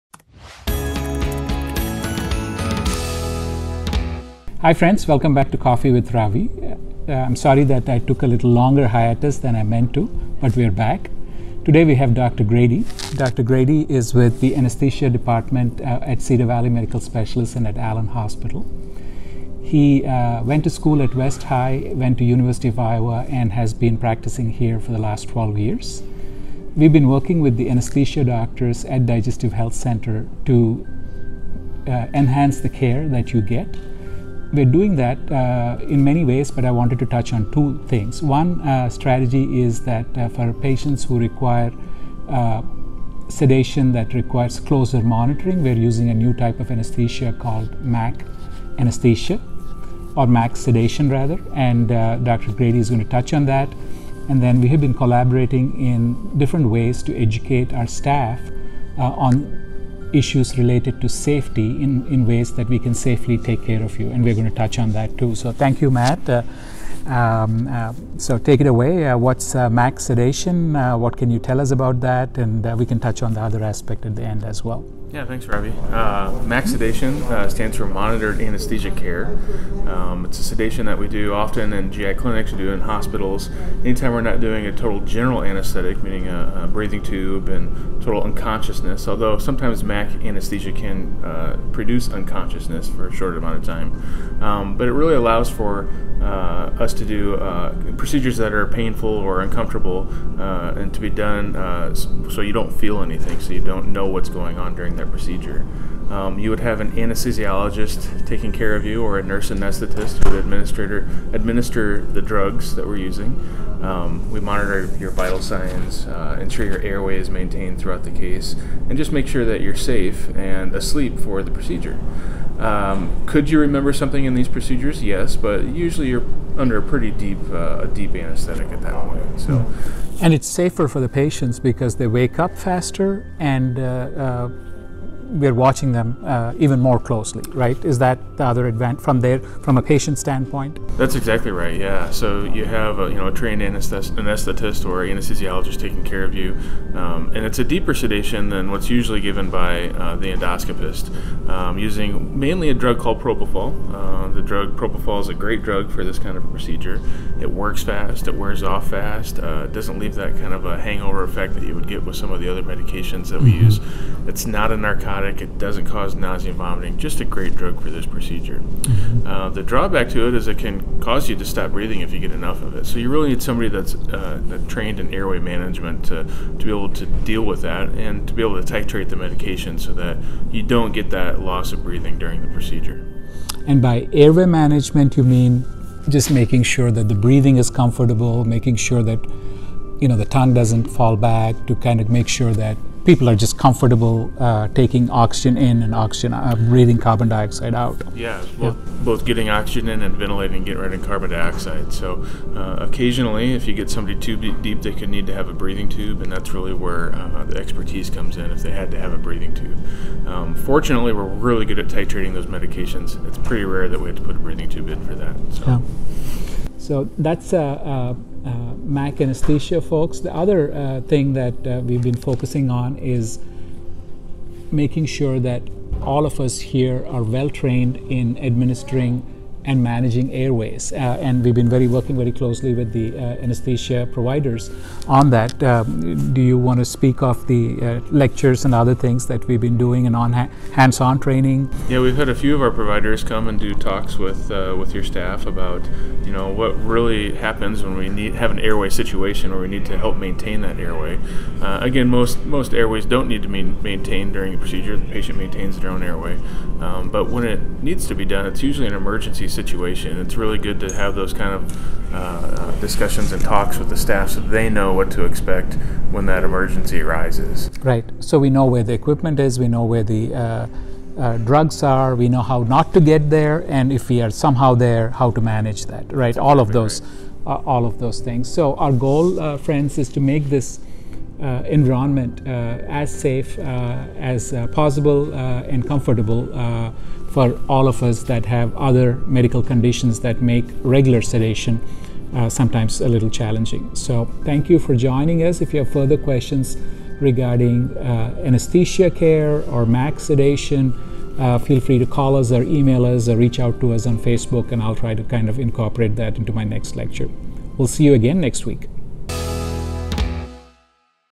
Coffee with Dr. Ravi #42: MAC Anesthesia
Welcome back to Coffee with Ravi. This week, we're talking to special guest Dr. Matthew Grady from Cedar Valley Medical Specialists' anesthesiology department about MAC anesthesia. We'll also discuss commonly used medications for MAC anesthesia along with safety during these procedures.